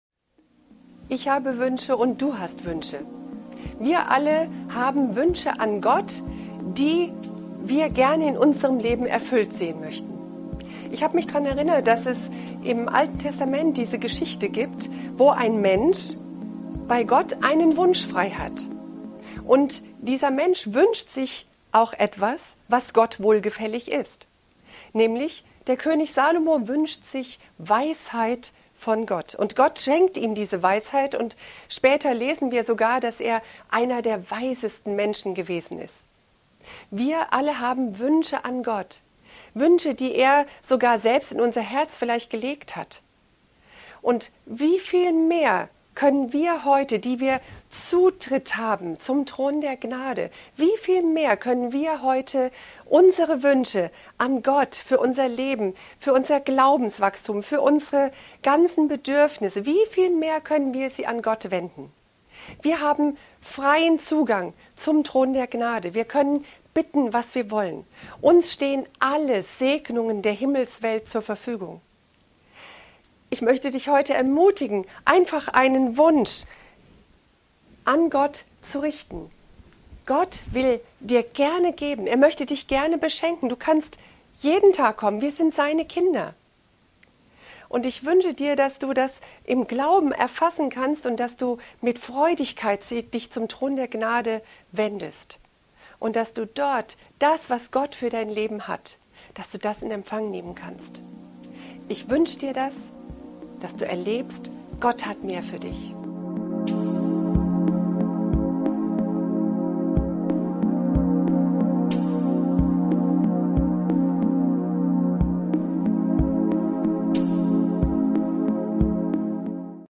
Wish
Jeder von uns hat Wünsche. Im Alten Testament hat Gott einem Menschen einen Wunsch freigegeben: „Bitte, was ich dir geben soll!". Also: Wünsche dir, was du willst, ich werde es dir erfüllen. Nicht schlecht, oder? Heute sieht die Sache jedoch ganz anders aus, als zur Zeit des Alten Testaments. Denn jetzt haben wir freien Zugang zum Thron der Gnade...

Die Person, der Gott einen Wunsch gewährte, war Salomo und er wünschte sich Weisheit:

In Gibeon erschien der HERR dem Salomo in einem Traum bei Nacht. Und Gott sprach: Bitte, was ich dir geben soll!
1.Könige 3,5

Im Neuen Testament spricht es uns Jesus ganz genauso zu:

Bittet, und es wird euch gegeben werden; sucht, und ihr werdet finden; klopft an, und es wird euch geöffnet werden!
Matthäus 7,7

Heute haben wir als Kinder Gottes freien Zugang zum Thron der Gnade und können Gott jederzeit um seine Segnungen bitten -- immer und immer wieder. Er gewährt uns nicht einen einzigen Wunsch, sondern unzählige. Uns stehen alle Segnungen der Himmelswelt zur Verfügung (Epheser 1,3). Das sind in erster Linie geistliche Segnungen für unser geistliches Leben -- und diese sind viel kostbarer als irdische Segnungen für unser irdisches Leben.

Ich möchte dich ermutigen, einfach einen Wunsch an Gott zu richten. Du darfst jederzeit zu ihm kommen wenn du sein Kind bist, an jedem Tag. Gott will dir gerne geben.

Gepriesen sei der Gott und Vater unseres Herrn Jesus Christus! Er hat uns gesegnet mit jeder geistlichen Segnung in der Himmelswelt in Christus
Epheser 1,3

Den Wunsch seines Herzens hast du ihm gewährt, und das Verlangen seiner Lippen nicht verweigert.
Psalm 21,3

Was ihr den Vater bitten werdet in meinem Namen, wird er euch geben ... Bittet, und ihr werdet empfangen, damit eure Freude völlig sei!
Johannes 16,23-24

Was immer wir bitten, empfangen wir von ihm, weil wir seine Gebote halten und das vor ihm Wohlgefällige tun.
1.Johannes 3,22